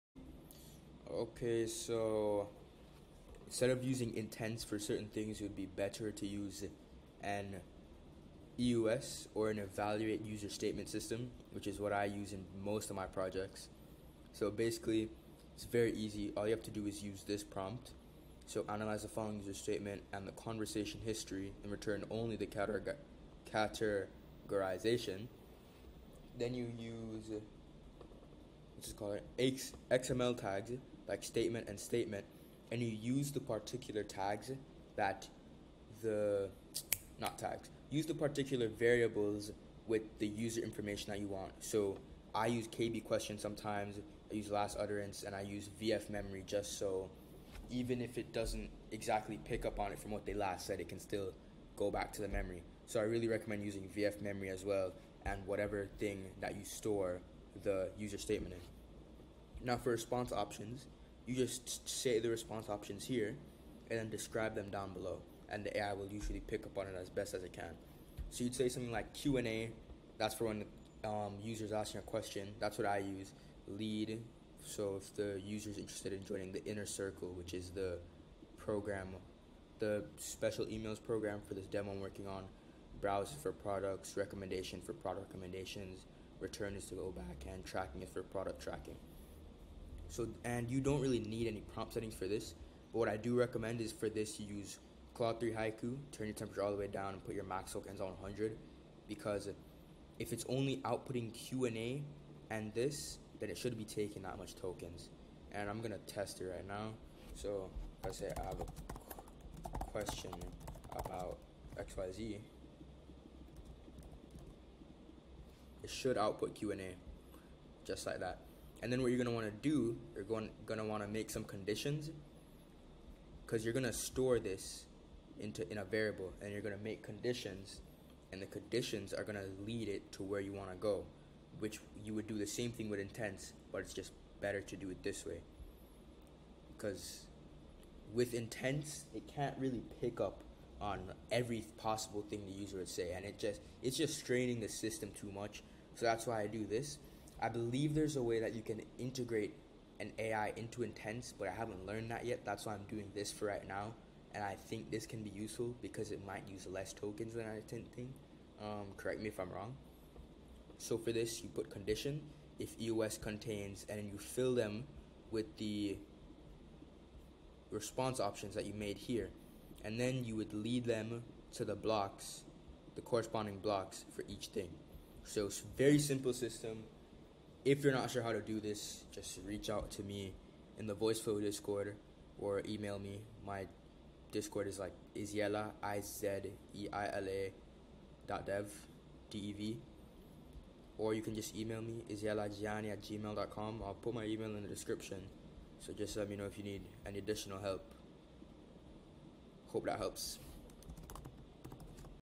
DON'T use intents in Voiceflow, do this instead
Stop using intents in voiceflow for directing a user based on a query. Not very useful.